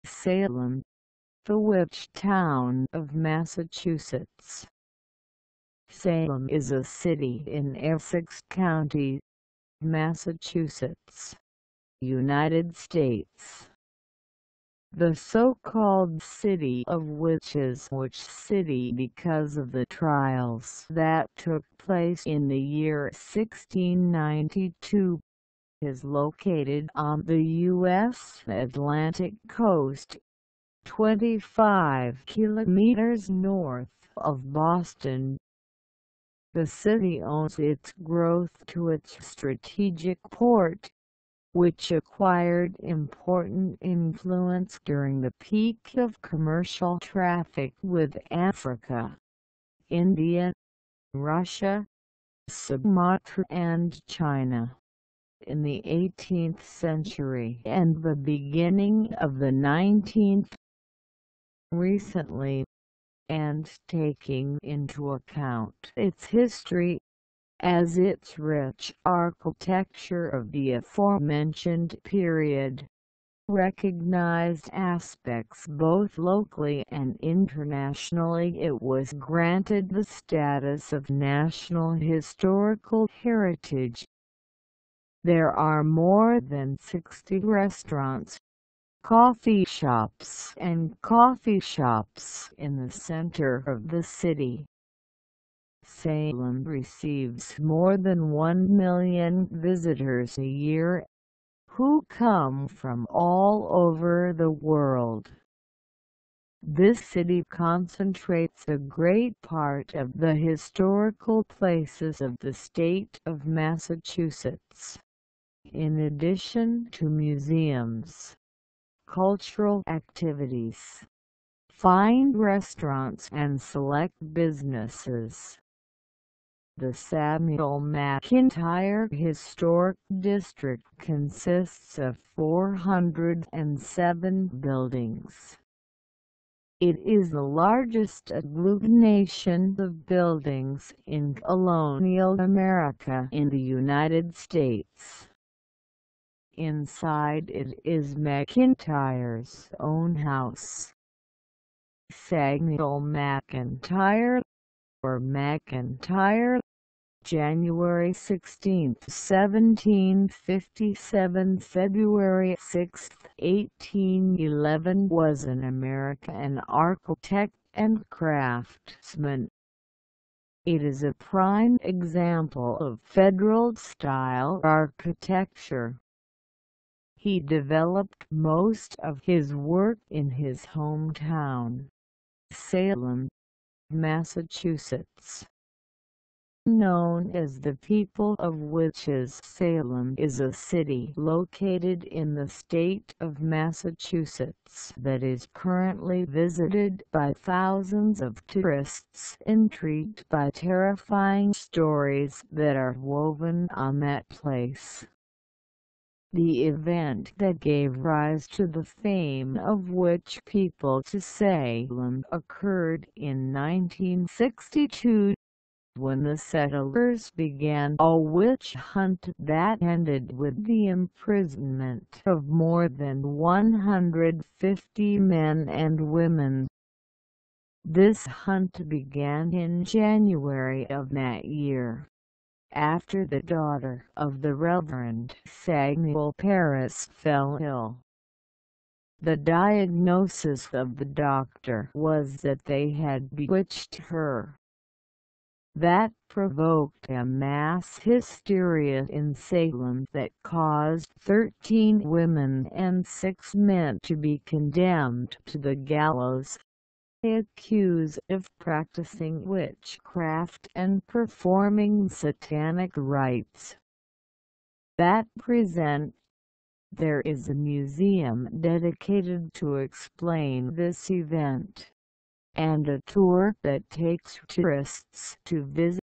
Salem, the witch town of Massachusetts
Salem is a city in Essex County, Massachusetts, United States.
The so-called "City of Witches" (Witch City) because of the trials that took place in the year 1692, is located on the US Atlantic coast, 25 km north of Boston.
The city owes its growth to its strategic port, which acquired important influence during the peak of commercial traffic with Africa, India, Russia, Sumatra and China; in the eighteenth century and the beginning of the nineteenth. Recently, and taking into account its history, as its rich architecture of the aforementioned period- (recognized aspects both locally and internationally), it was granted the status of National Historical Heritage. There are more than 60 restaurants, coffee shops and coffee shops in the center of the city.
Salem receives more than one million visitors a year, who come from all over the world. This city concentrates a great part of the historical places of the state of Massachusetts, in addition to museums, cultural activities, fine restaurants and select businesses.
 The Samuel McIntyre Historic District consists of four hundred and seven buildings. It is the largest agglutination of buildings in colonial America in the United States. Inside it is McIntyre's own house. Samuel McIntyre, or McIntire, (January 16, 1757 February 6, 1811) was an American architect and craftsman. It is a prime example of federal style architecture. He developed most of his work in his hometown, Salem, Massachusetts.
Known as "The people of witches", Salem is a city located in the state of Massachusetts that is currently visited by thousands of tourists intrigued by terrifying stories that are woven on that place.
The event that gave rise to the fame of "witch people" to Salem occurred in 1962, when the settlers began a witch hunt that ended with the imprisonment of more than 150 men and women.
This hunt began in January of that year, after the daughter of the Reverend Samuel Parris fell ill. The "diagnosis" of the doctor was that they had "bewitched" her.
That provoked a mass hysteria in Salem that caused 13 women and 6 men to be condemned to the gallows, accused of practicing witchcraft and performing satanic rites.
At present, there is a museum dedicated to explain this event, and a tour that takes tourists to visit the sites related to the "witch hunt".